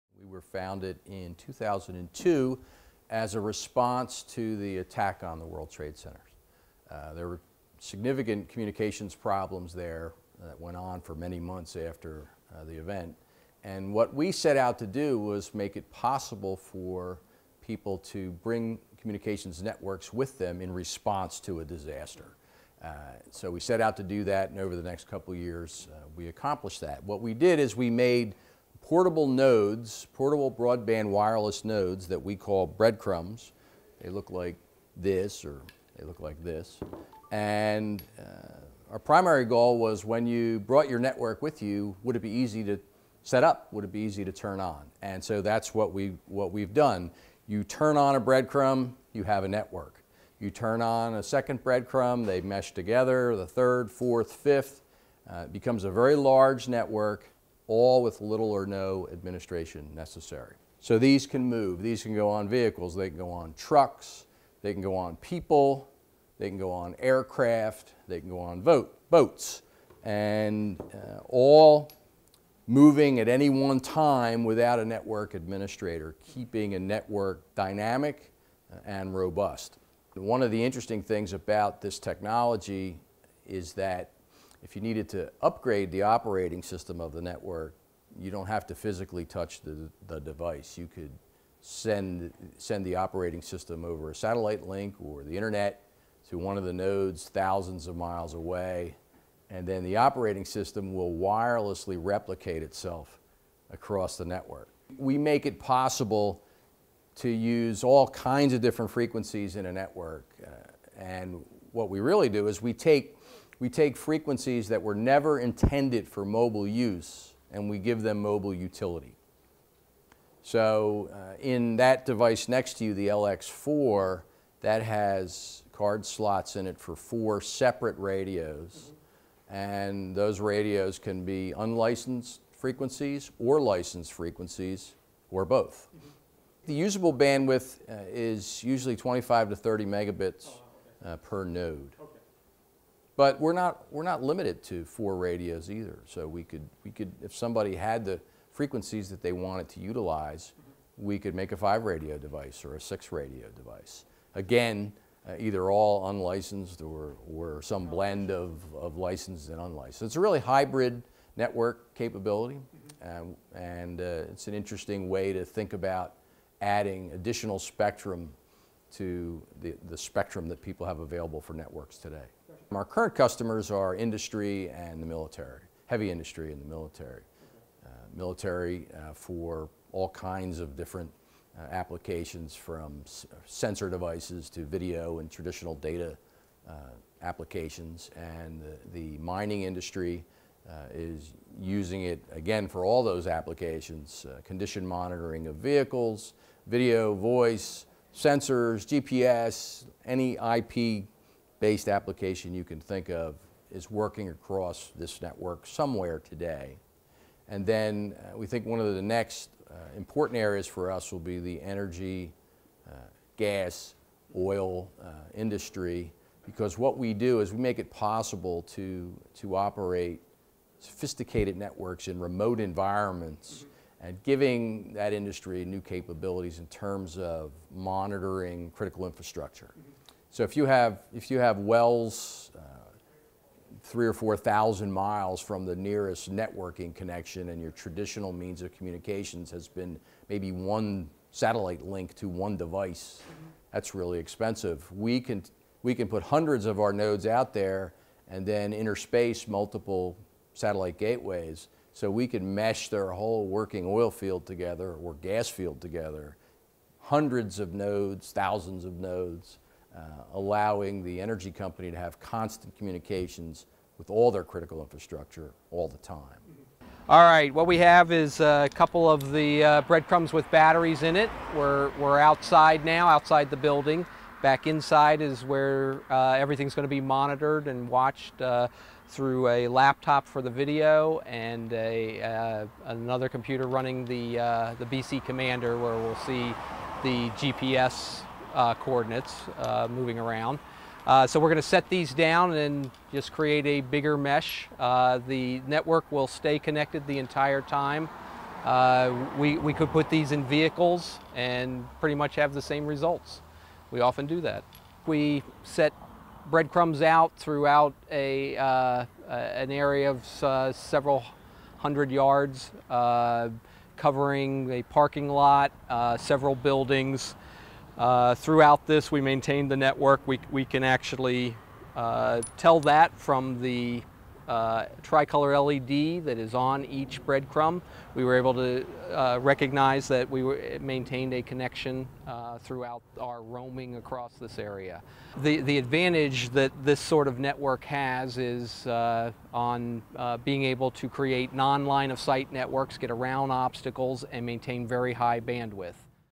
Philly Tour: Rajant Corporation
RCR Wireless meets with Robert J. Schena, Chairman, CEO & Co-founder and Paul Hellhake, CTO & Co-founder, Rajant Corporation. Based in Malvern, PA just outside of Philadelphia the company is responsible for the development of a wireless networking node dubbed the "Breadcrumb." With customers including heavy industry and the military the Breadcrumb allows users to create a broad robust network using both licensed and unlicensed frequencies by creating a mesh between multiple Breadcrumb devices.